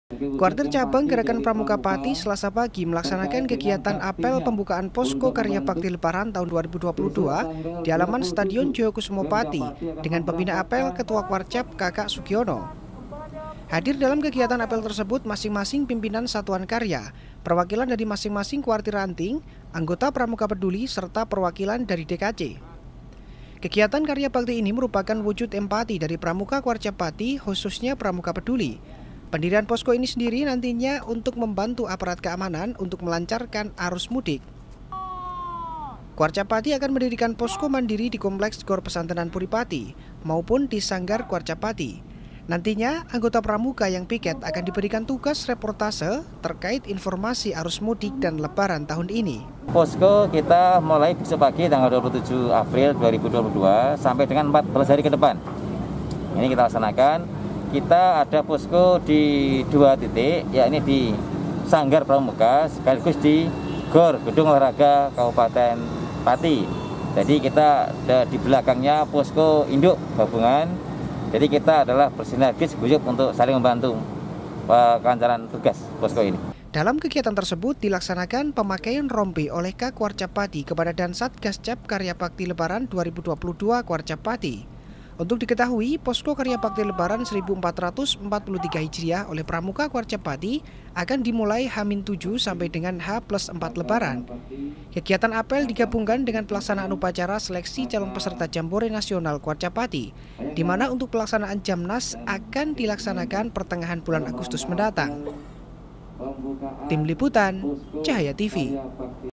Kwarcab Pati Gelar Apel Pembukaan Posko KBL Th. 2022
Kwartir Cabang Gerakan Pramuka Pati, Selasa Pagi, melaksanakan kegiatan apel Pembukaan Posko Karya Bhakti Lebaran Tahun 2022, di halaman Stadion Joyokusumo Pati, dengan Pembina Apel Ketua Kwarcab, Kakak Sugiyono.